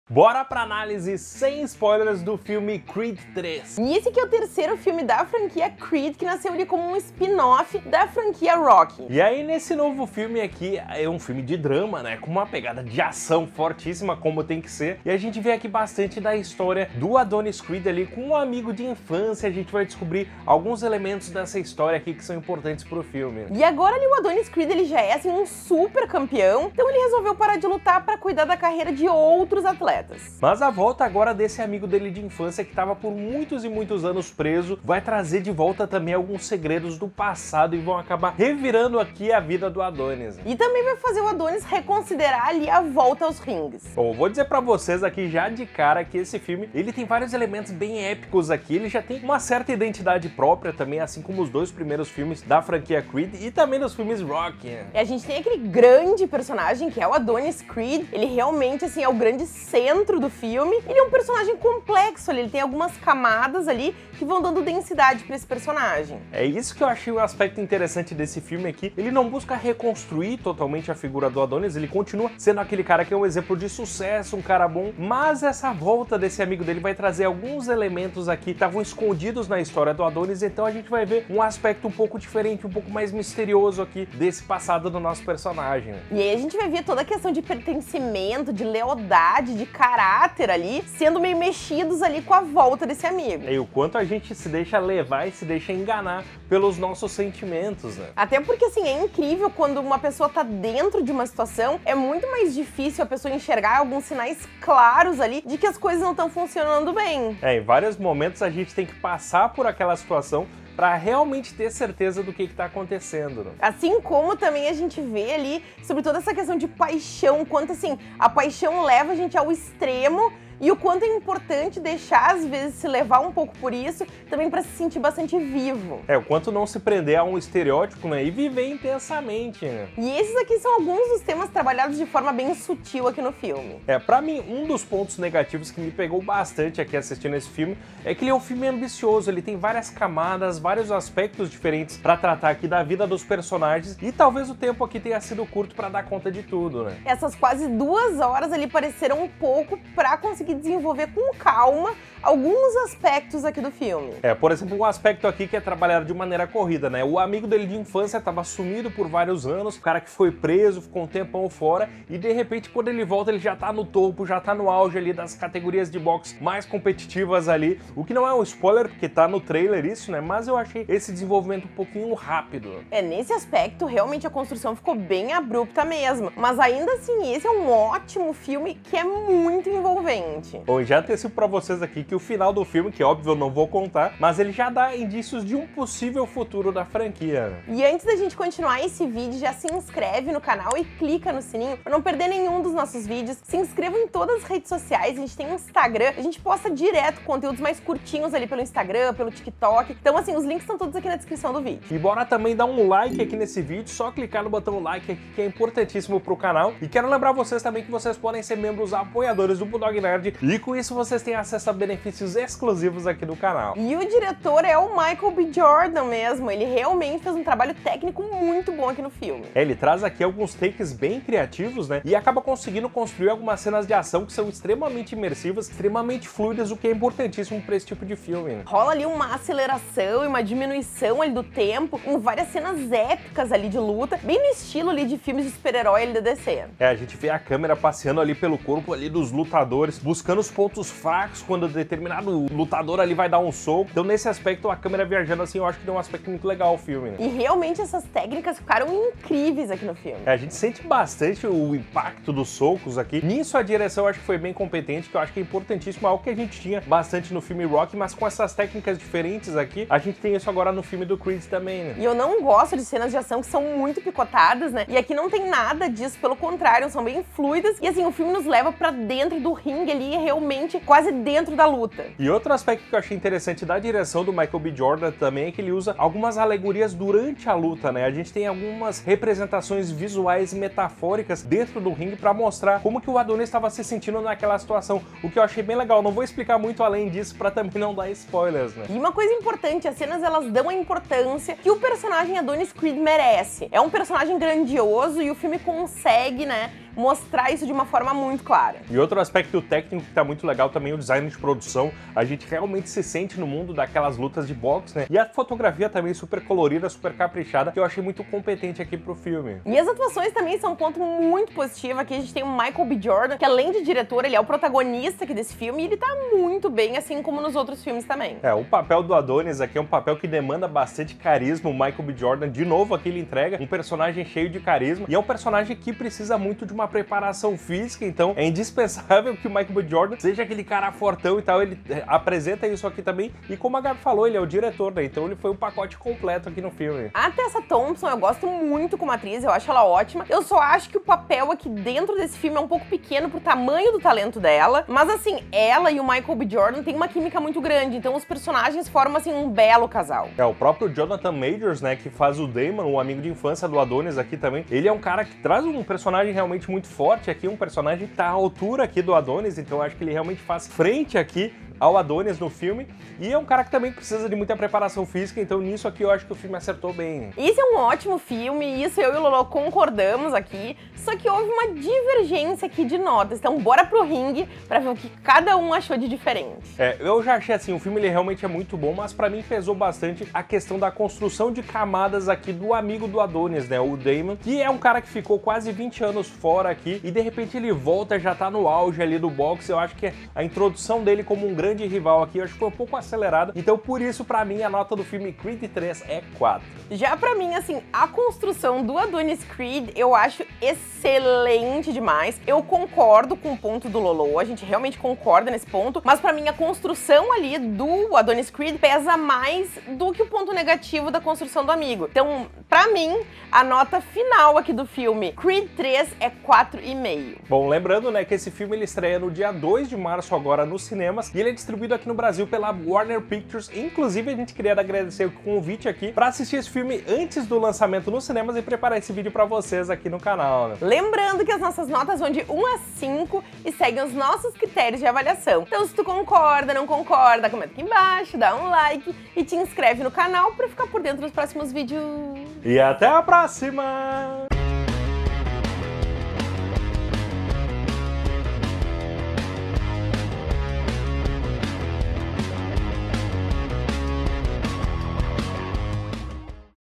CREED III | Vale a pena assistir? | análise sem spoilers
Análise sem spoilers do filme "Creed III".

⭐ID da PSN: bulldog_nerd_
⭐Gamertag do Xbox: BulldogNerdXbox
⭐Código de amigo Nintendo: SW-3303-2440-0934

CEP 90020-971
Porto Alegre-RS